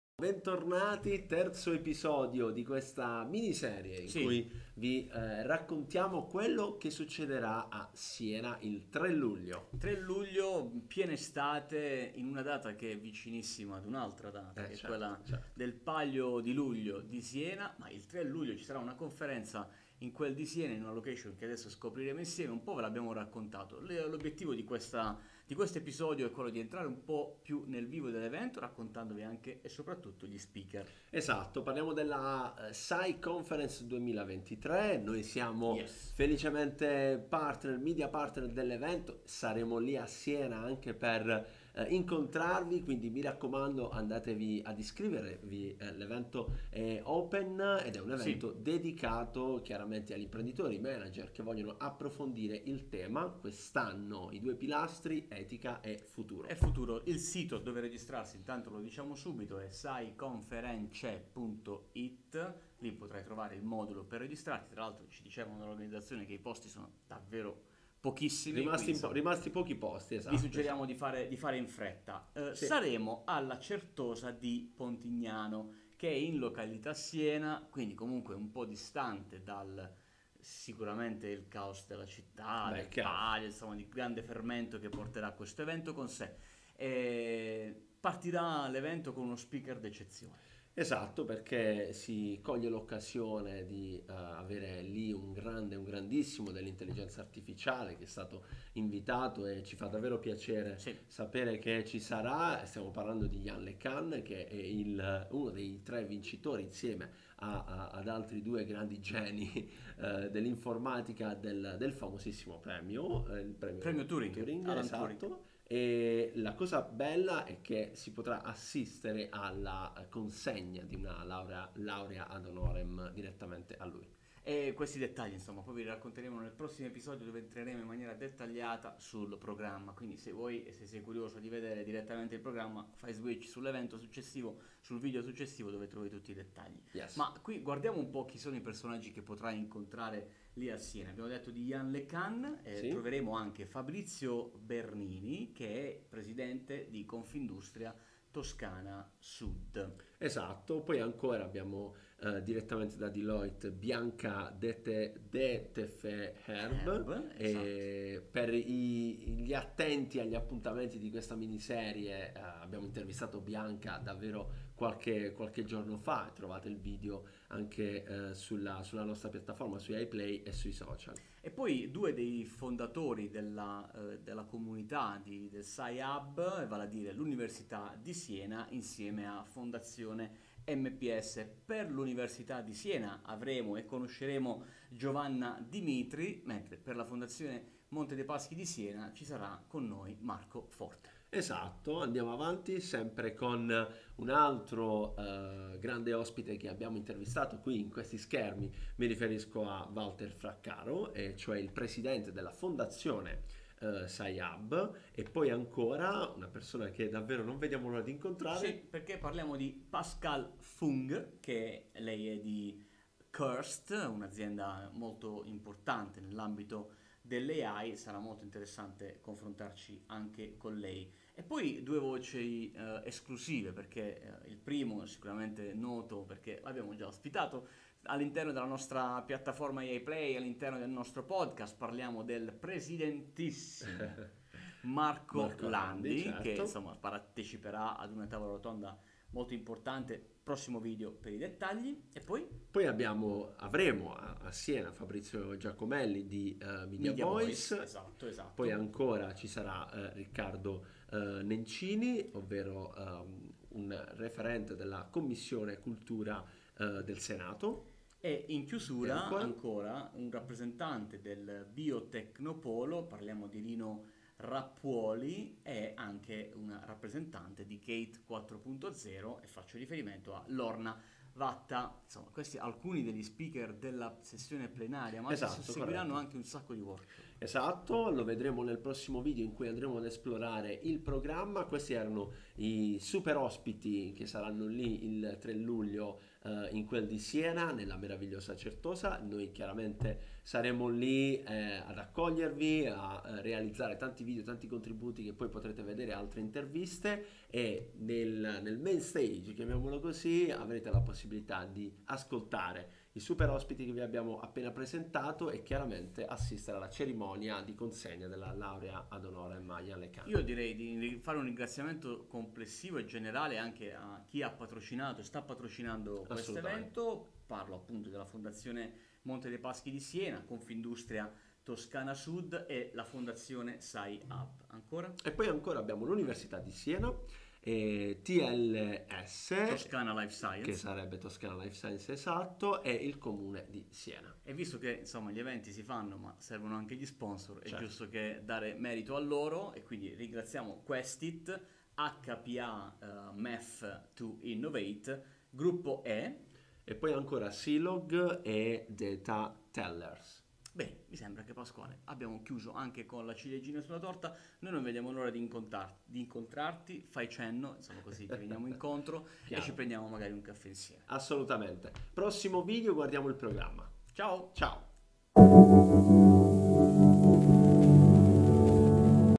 SAI CONFERENCE 2023 | Presentiamo gli Speaker, Sponsor e Partner
Ci vediamo a Siena alla
La conferenza annuale del SAIHub Siena Artificial Intelligence Hub che si terrà il Lunedì 3 luglio 2023 presso la Certosa di Pontignano (SI).

Pasquale Viscanti e Giacinto Fiore saranno all'evento per intervistare tutti i principali protagonisti. Tutti i video li troverai su AI Play.

La SAICONFERENCE23 avrà quest’anno come filo conduttore due temi portanti: etica e futuro. Etica per le scelte condotte dalle macchine, per l’introduzione di questa tecnologia a contatto con l’uomo in più scenari applicativi; futuro come competizione, come sostenibilità e come opportunità.